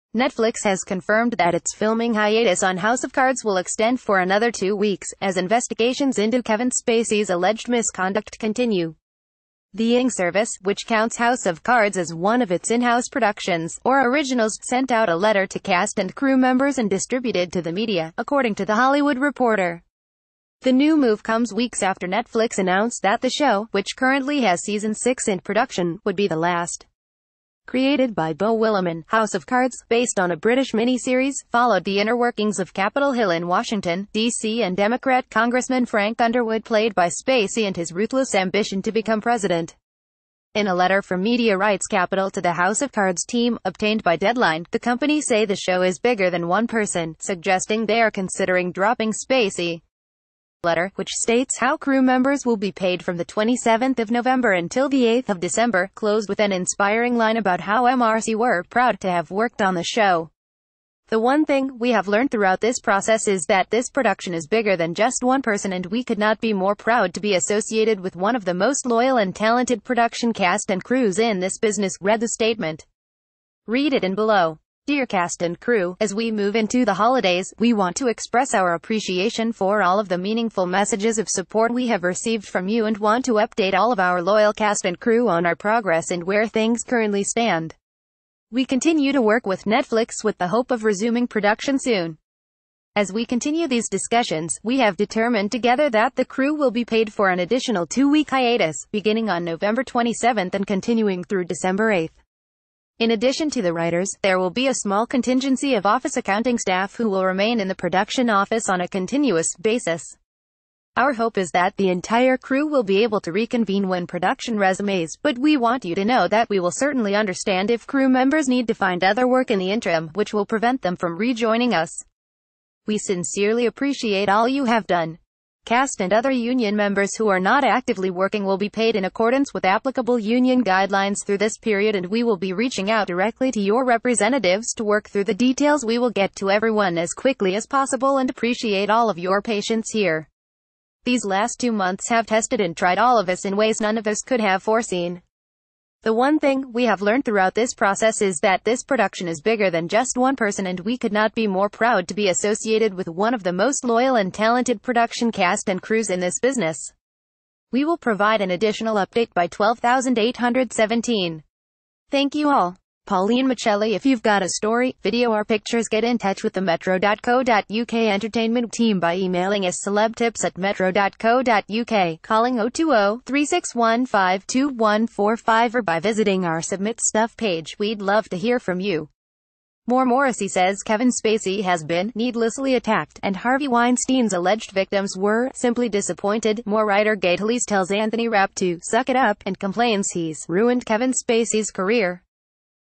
Latest News 365 - expansion card's home interrupted between kevin spacey allegations
Latest News 365 - Latest News 365 -  expansion cards home interrupted between kevin spacey allegations

Netflix has confirmed that its filminghiatus on House Of Cards will extend for another two weeks, as investigations into Kevin Spacey’s alleged misconduct continue. The ing service, which counts House Of Cards as one of its inhouse productions, or Originals, sent out a letter to cast and crew members and distributed to the media, according to The Hollywood Reporter. The new move comes weeks after Netflix announced that the show, which currently has season 6 in production, would be the last. Crea...